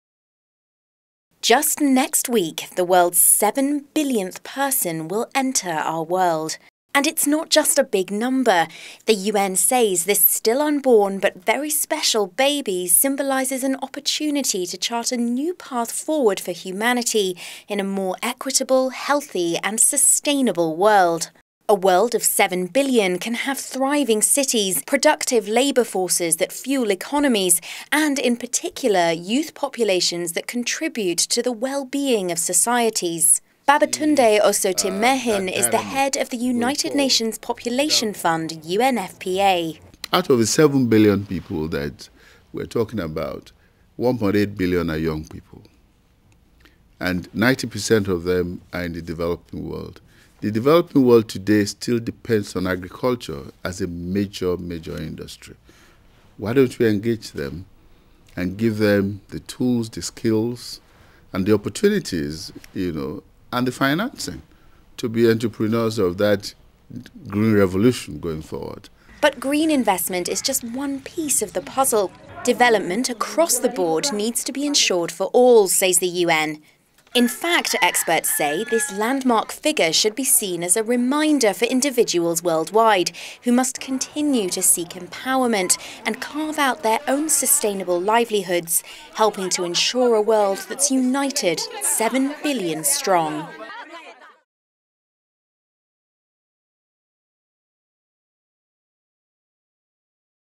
As global population hits 7 billion, UN urges action to ensure development for all
26 October 2011 -- With the world population projected to reach 7 billion in five days' time, actions taken now will decide whether the future will be healthy, sustainable and prosperous or marked by inequalities, environmental decline and economic setbacks, according to a United Nations report issued today.
The world must seize the opportunity to invest in the health and education of its youth to reap the full benefits of future economic development or else face a continuation of the sorry state of disparities in which hundreds of millions of people in developing nations lack the most basic ingredients for a decent life, UN Population Fund (UNFPA) Executive Director Babatunde Osotimehin said in the foreword of the study.